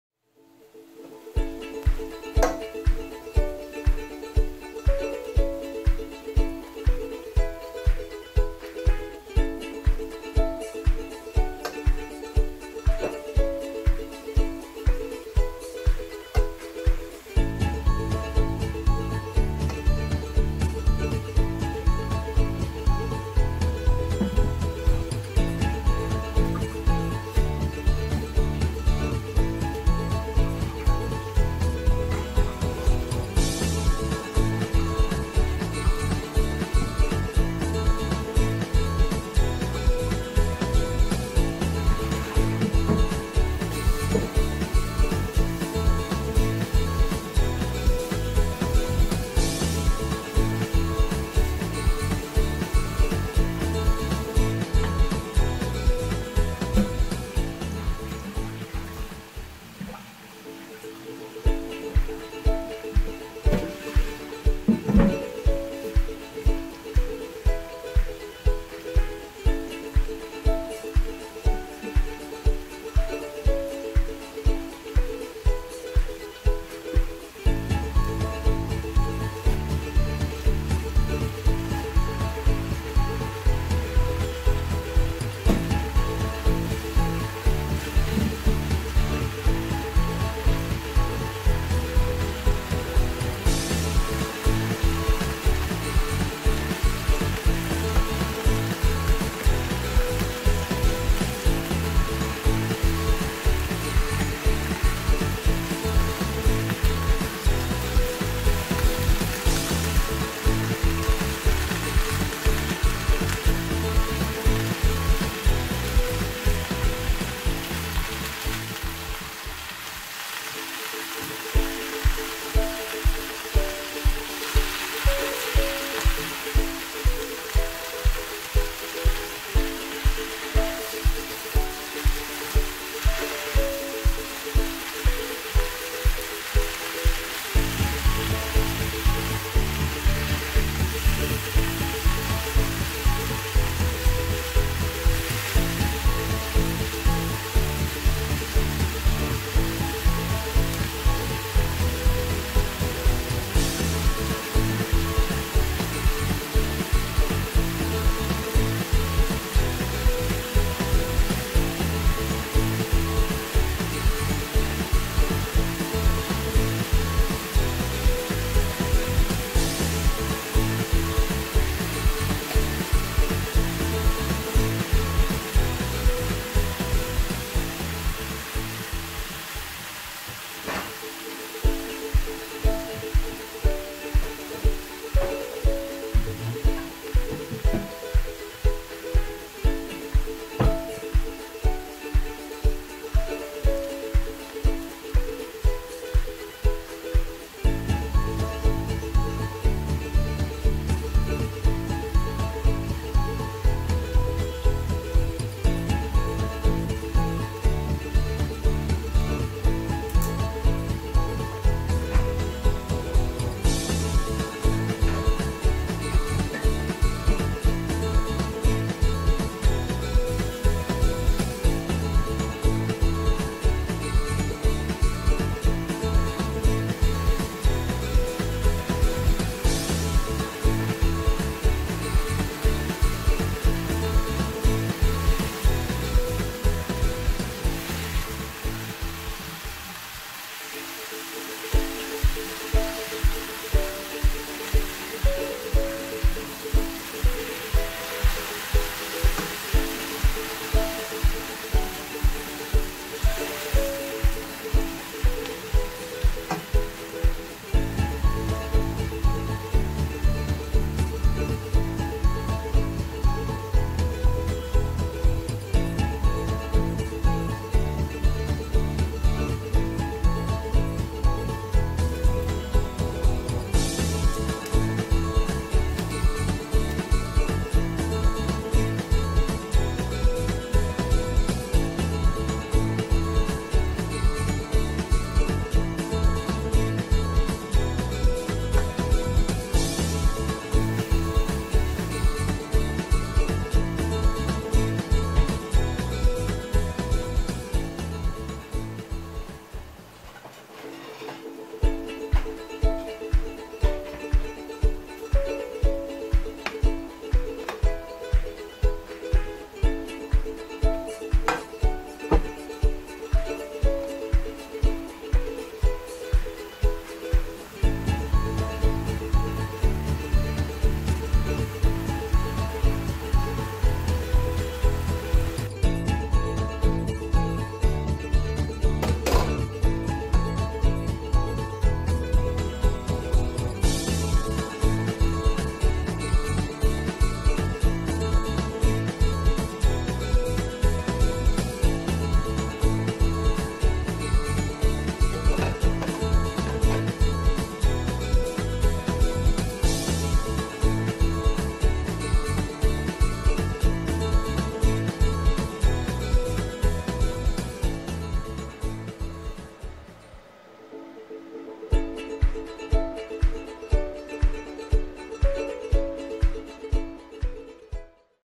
the easy way to cook mix mushrooms with choy sum
mushrooms is very delicious can mix  with any kind of vegies you can make,as a salad as,well